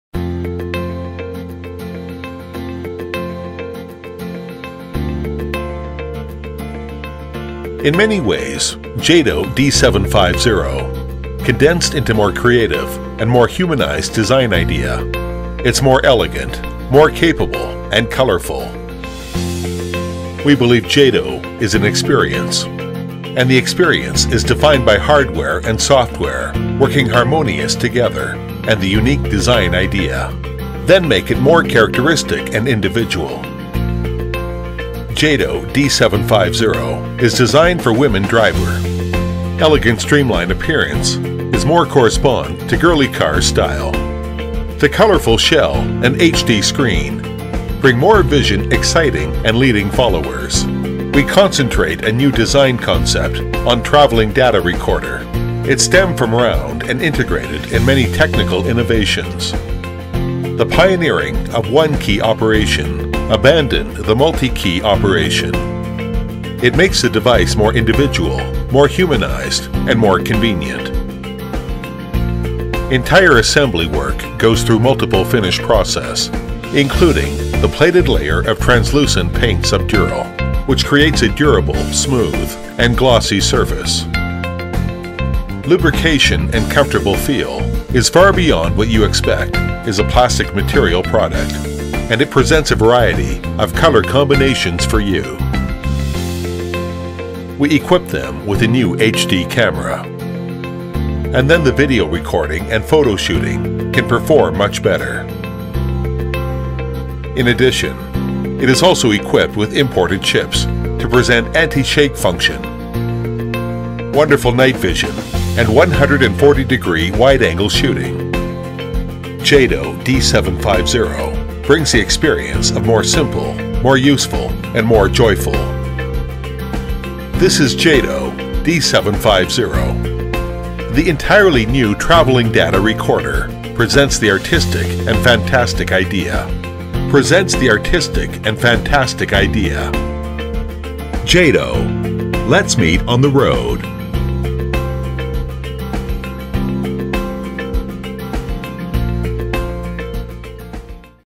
JADO-D750 Exclusive colorful Ball Shape Feminization Adjustable 1080P HD DVR Dash Camera
If interested,pls refer to Amazon

*JADO D750 -- Car Dash cam for female customization.First public appearance in Germany,amaze all in Europe.
*Special ball shape design,drive the new trend in the market.
*A full HD video recorder with a 5MP camera sensor that takes brilliant videos at 30 fps with H.264 compression for a smaller size.
*Easy install and use for this DVR,one key to a number of  innovative  operation.
Features:
★ More colorful:Diversity,not just women.
★ Recording resolution:1920x1080 pixels.Record in High Definition.
★ 140 Degree Wide Angle / 2.7" LCD Screen - Rotates 360 degrees
★ Automatic Emergency recording for collision detection.
★ Seamless loop-cycle video & audio recording.Your Eyewitness That Never Blinks
★ Advanced Spraying process:use British senior custom paint,as automobile paint quality.
★ If you choose it as a Christmas present,it would be a amazing surprise for them.